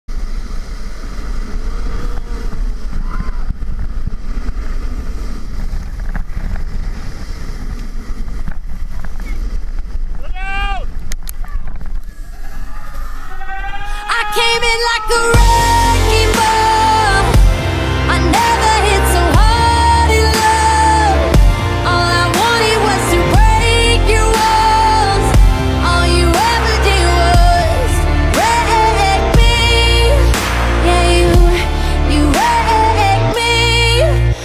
Snowtubing at Whitetail
People really shouldn't hang around at the end of the hill. Note that there are actually two little kids- Watch to the end and decide for yourself what happened to the second one.